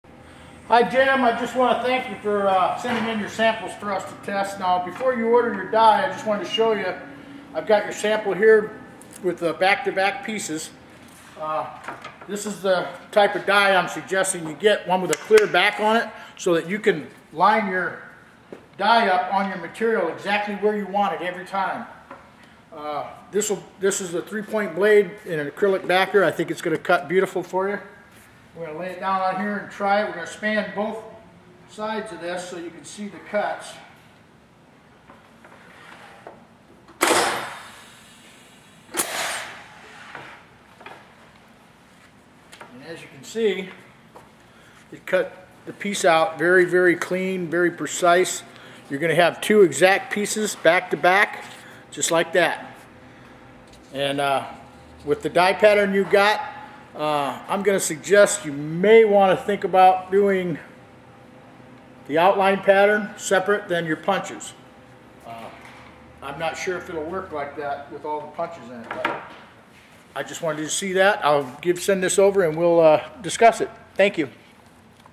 Tippmann Clicker 700 steel rule die cutting press cutting vinyl
Don Robertson demonstrates the Tippmann Clicker Steel Rule die cut machine cutting vinyl using a .937 steel rule cutting die.